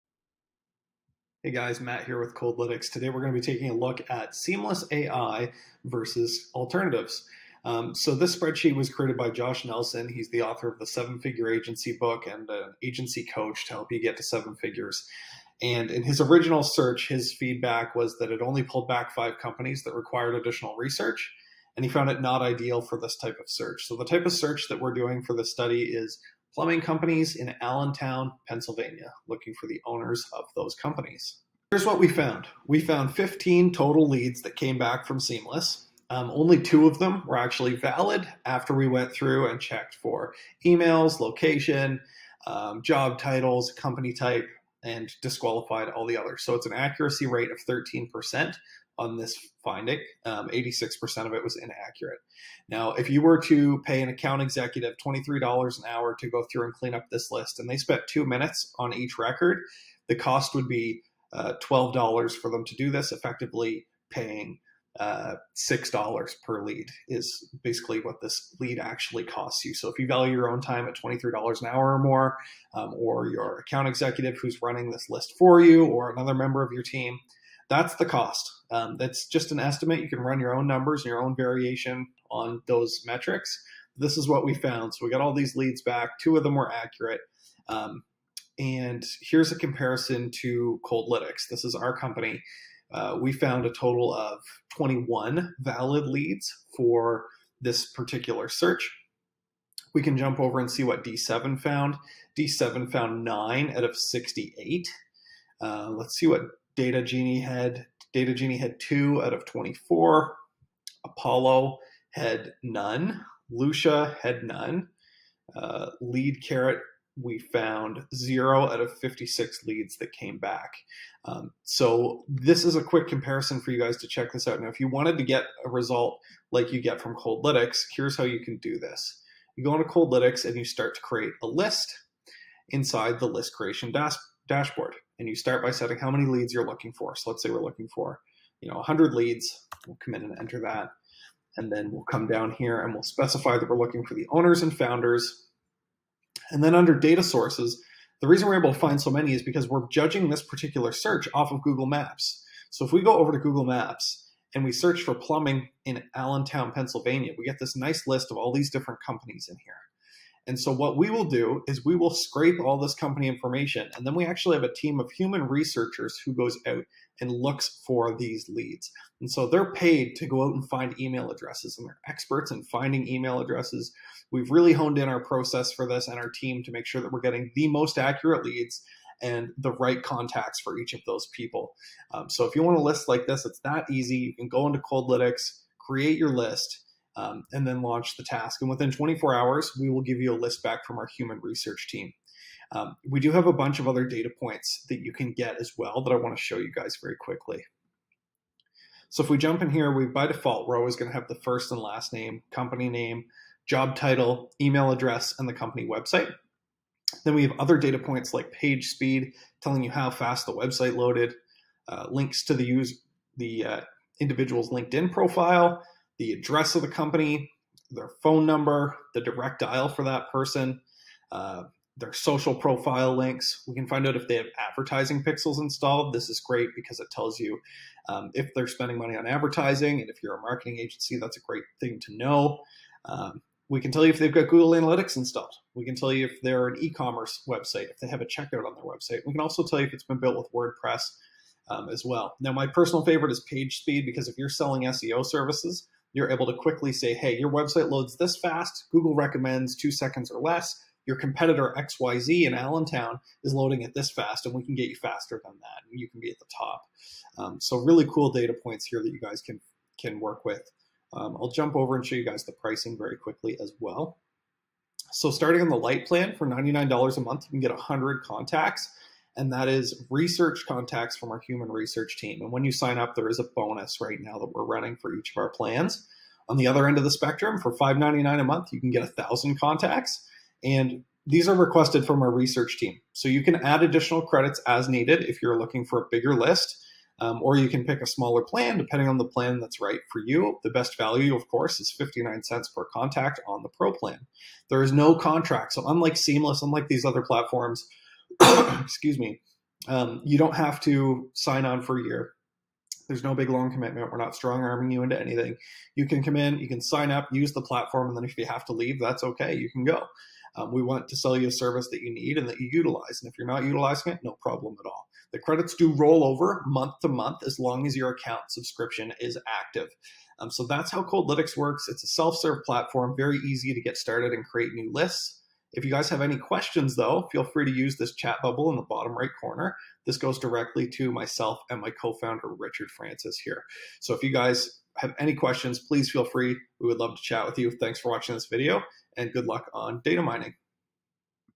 Seamless AI Review | Compare with Apollo, Data Axel, Lead Carrot, Lusha and Coldlytics
Seamless AI Reviews are all over the charts, from 1.5 stars on Trustpilot to over 4 stars on G2, it's hard to know if Seamless AI is worth it.

We did a live test of Seamless using a search for Plumbing companies in Allentown, PA.

This comparison was originally done by Josh Nelson, author of The 7 Figure Agency book and coach to hundreds of marketing agencies.

Our results showed us less than 10% of what were able to find using Coldlytics, and an interesting comparison to other top data providers like us.

00:00 Seamless AI Review
01:28 Comparison to Coldlytics
01:41 Comparison to D7 Lead Finder and Data Axel
01:52 Comparison to Apollo, Lusha, and Lead Carrot
02:09 How to Build A List
02:32 Find Leads on Google
03:18 Data Points
04:20 Coldlytics Pricing